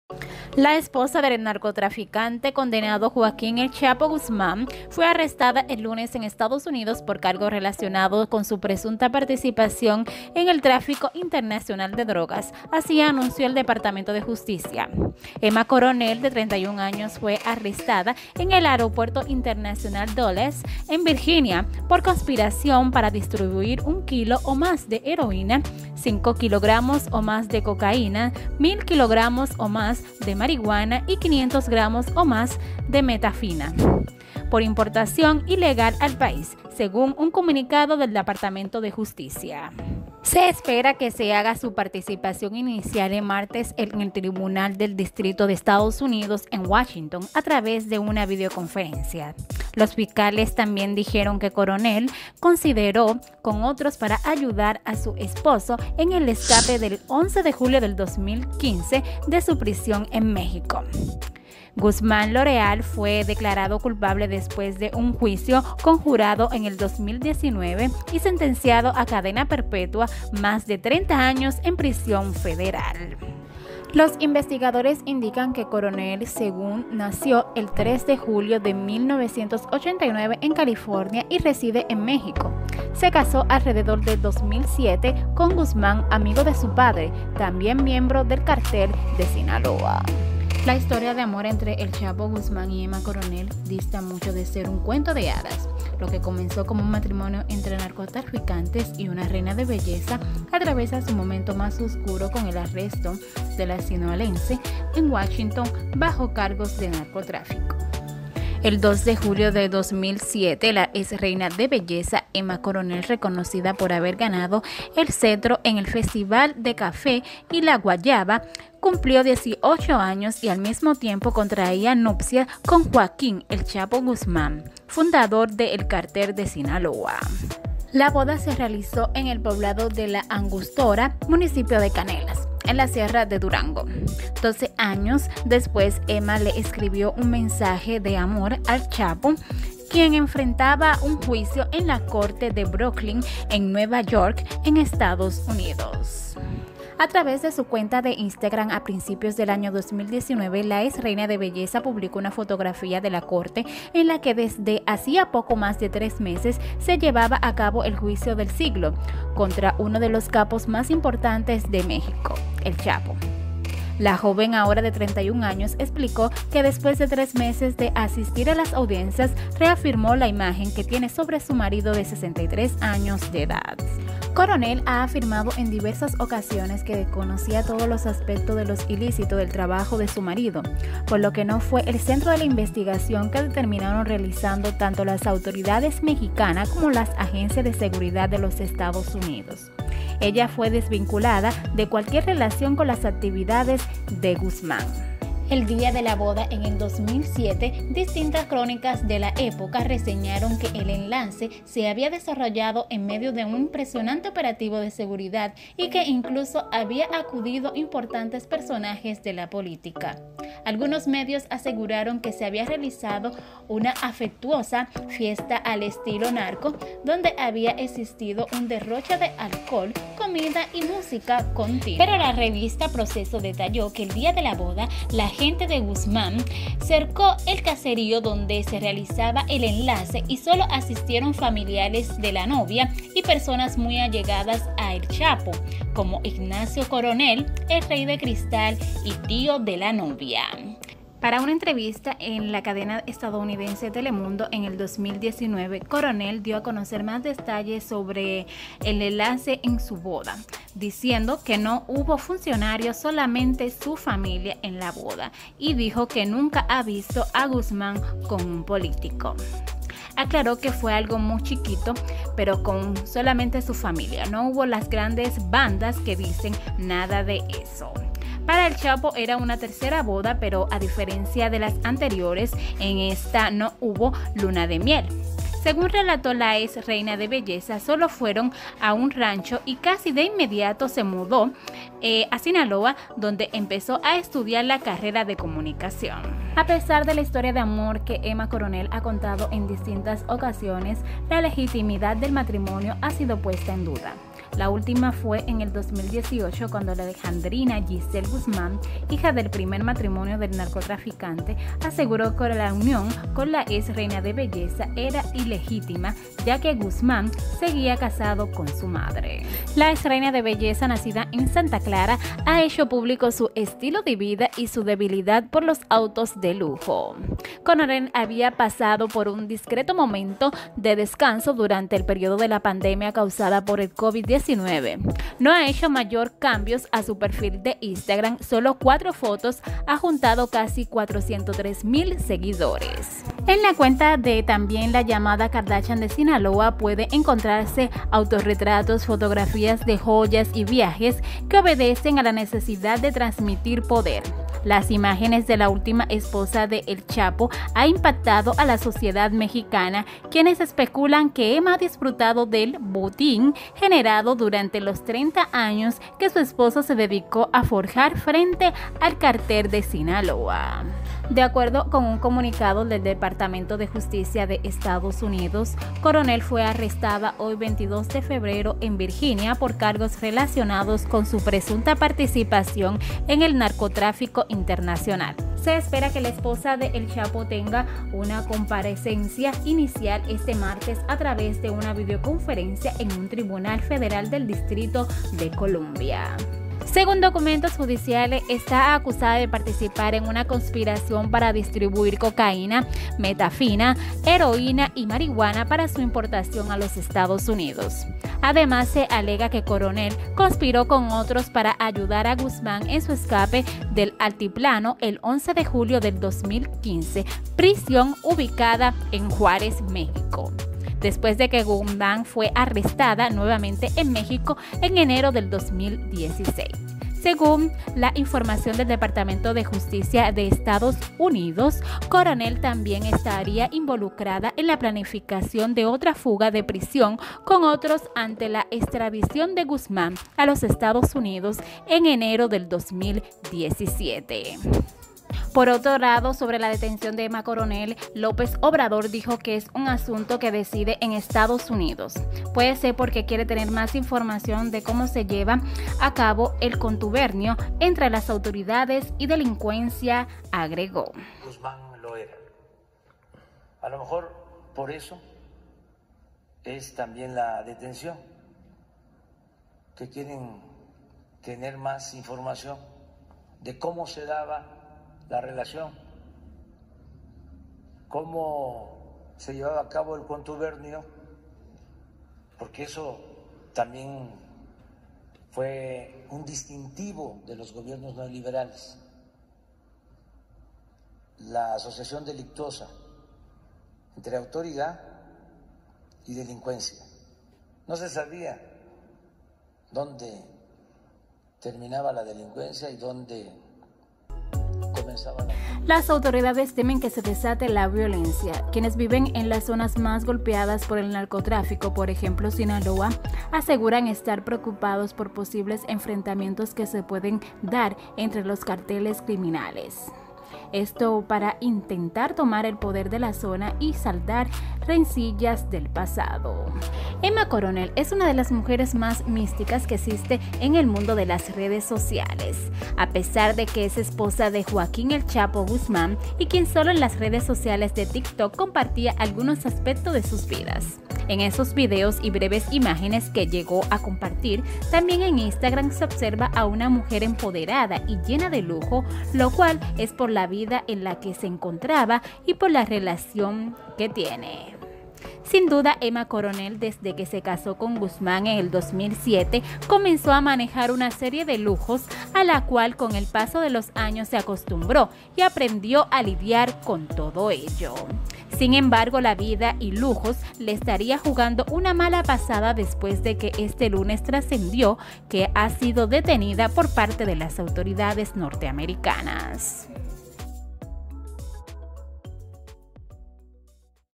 Arrestan a Emma Coronel, esposa de "El Chapo" | Los lujos que tenía Emma
Arrestan a la esposa del Chapo Guzmán, Emma Coronel por narcotráfico internacional